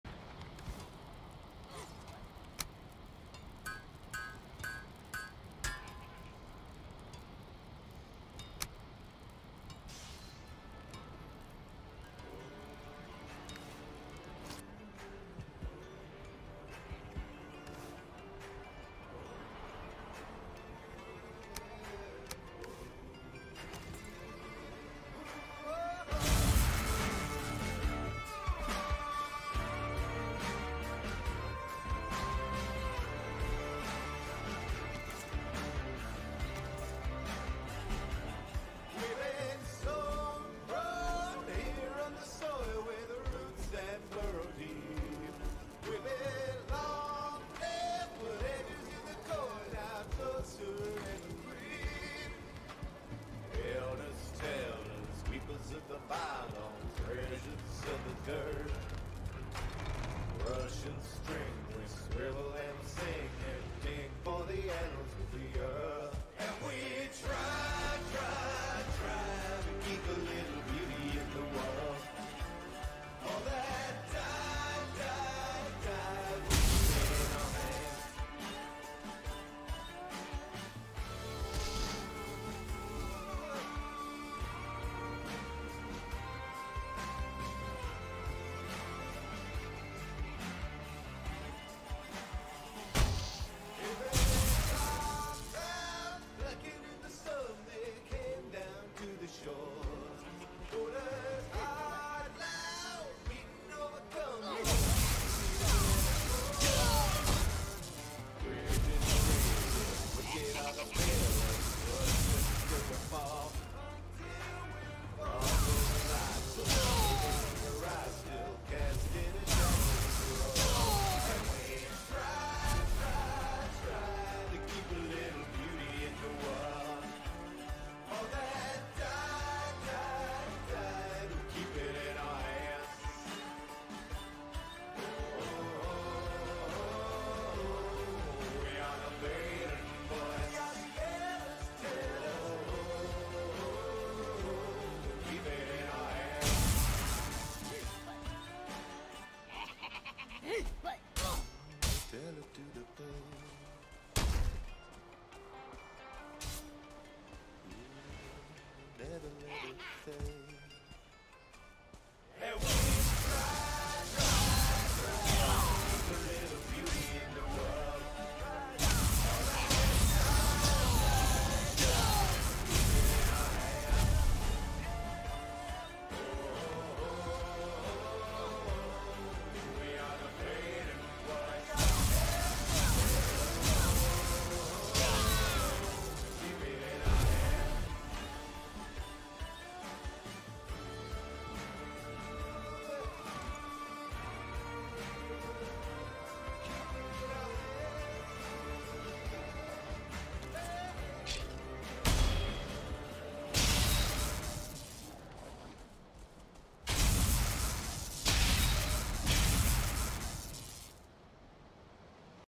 Crafting and testing the Himmin Afl. Is this the best polearm weapon in the game?
If Thor used a polearm weapon this would be his!